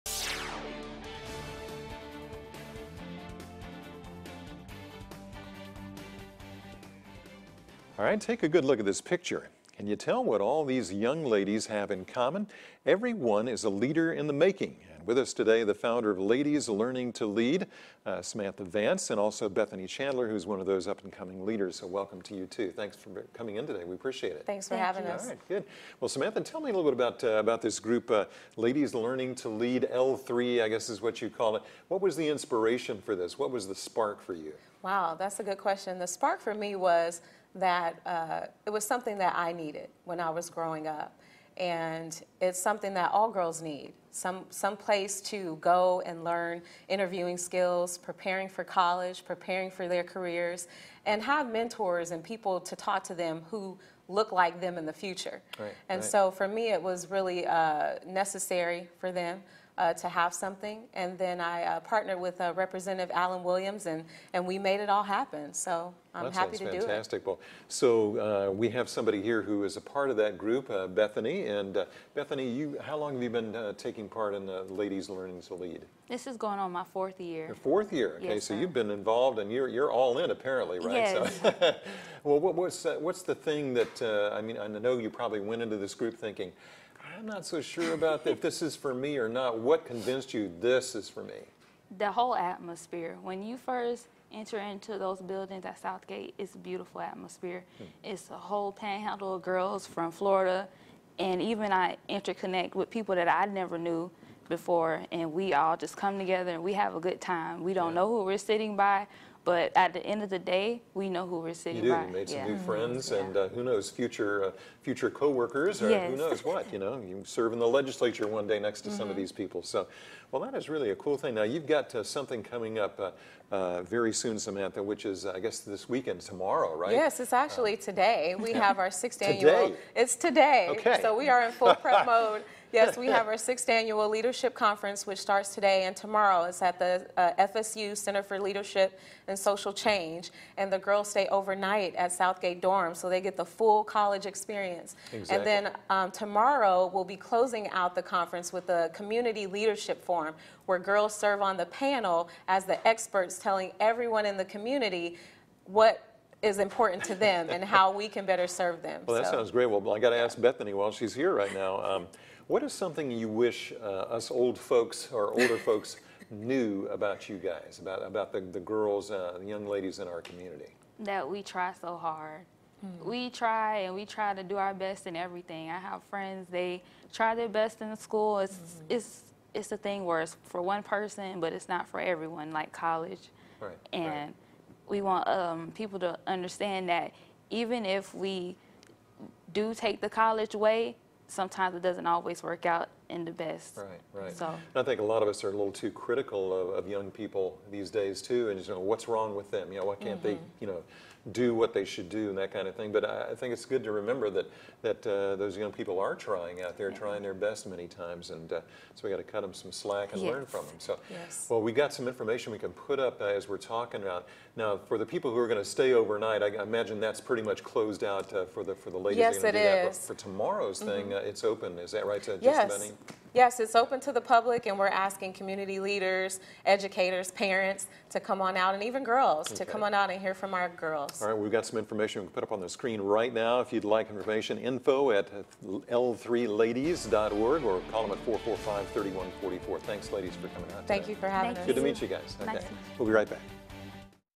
L3 Developing Young Female Leaders
Ladies Learning to Lead (L3) founder and Executive Director Samantha Vance talks of the 6th Annual Leadership Conference and Girls Community Leadership Forum at FSU. Bethany Chandler joins her with insight on how L3 has impacted her life and what adults need to know to help young people.